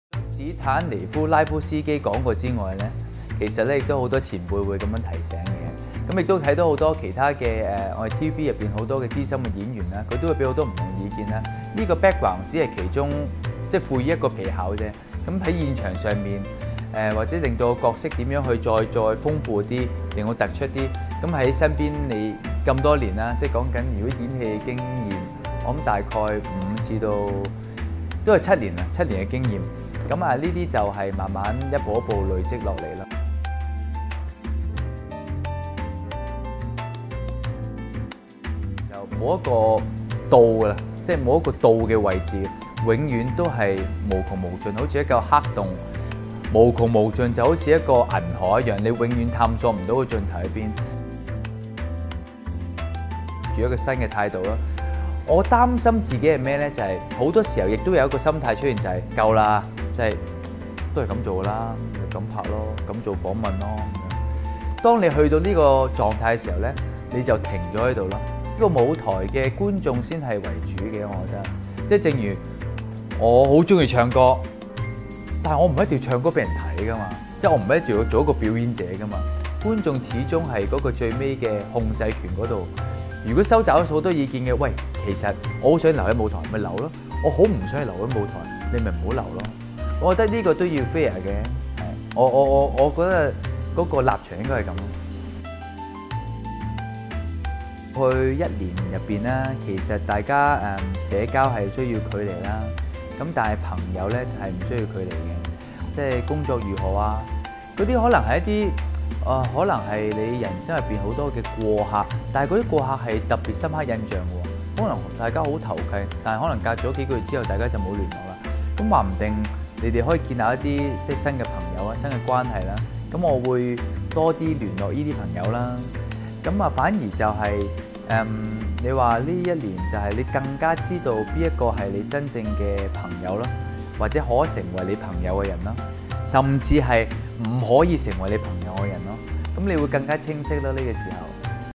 【CoverStory Issue007】洪永城
對於洪永城主持旅遊節目，其功力是無庸置疑。而在演戲方面，其人生歴練漸多，讓他的思路更清𥇦，只要觀眾還想他留在這舞台上，洪永城便願意不斷探索，給大家認識多樣的他。

鳴謝
Text&Editor Suki
Photo Leozting
Art Yiu
Video 小鮮肉
Makeup Miffy Ng
Wardrobe Ermenegildo Zegna、 Diesel

🔵 Facebook WeTogetherHK
🟤 Instagram TogetherMagazineHK
🔴 YouTube Together Magazine HK